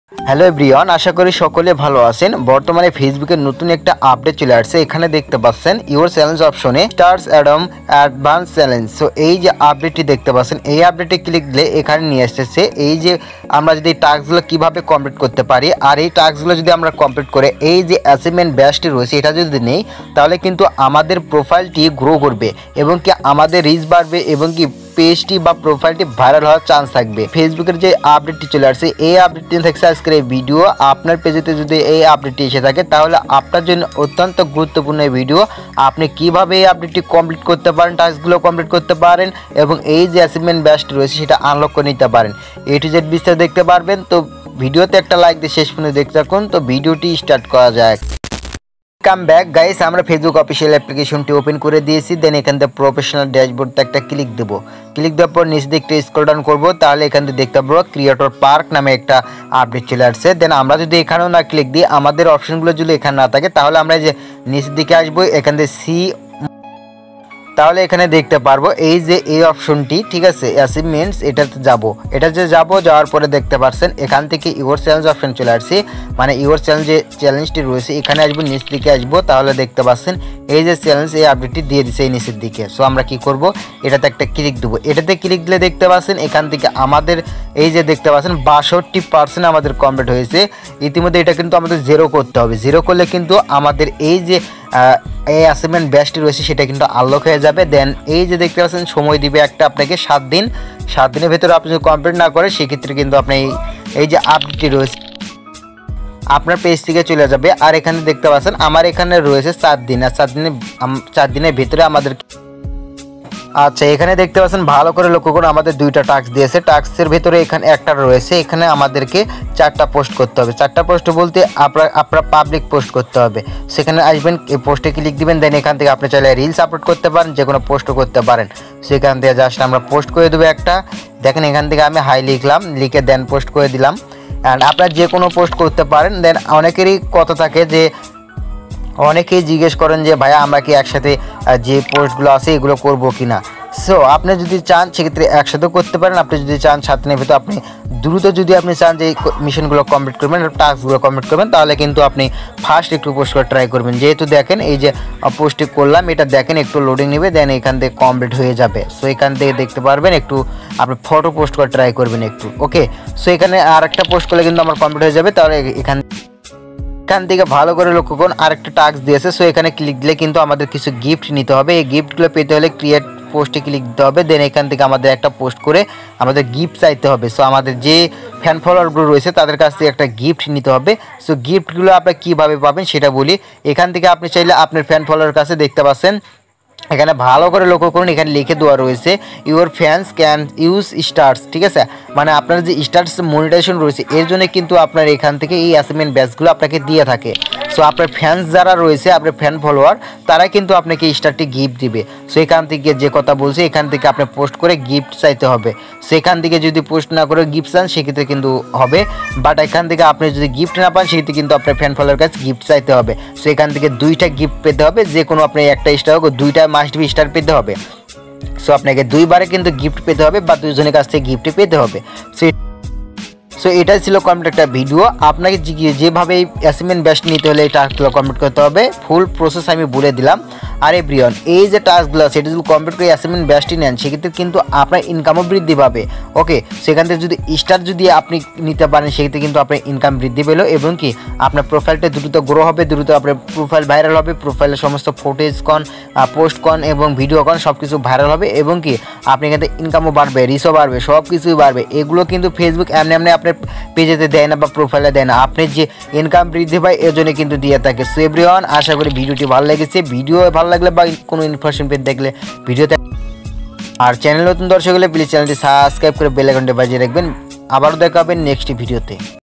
stars autumn adventure challenge facebook | star intermediate challange facebook | fb new update
In this video I have discussed facebook new update stars autumn adventure challenge, if you like the video and subscribe the channel stay tuned to this channel, to get the next video first.

Free music